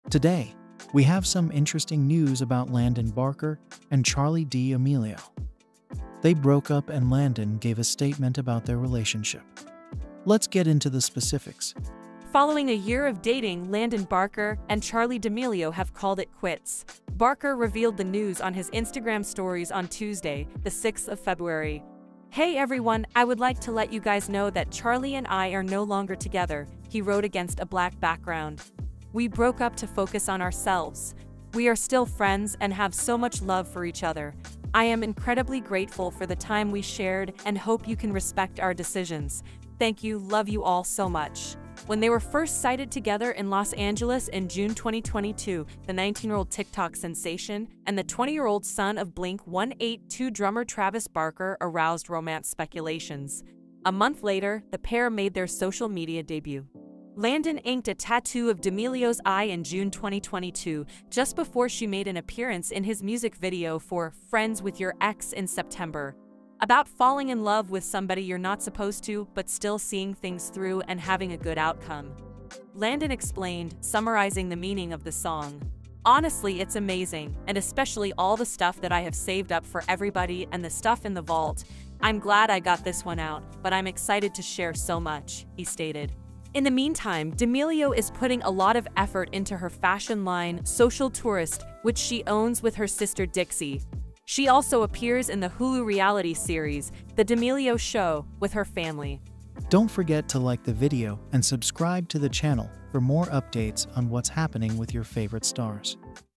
Landon Barker Spoke About Breakup With Charli D'Amelio
Landon Barker and Charli D'Amelio have called it quits.  Barker revealed the news on his Instagram Stories on Tuesday, the 6th of February.

Apple AirPods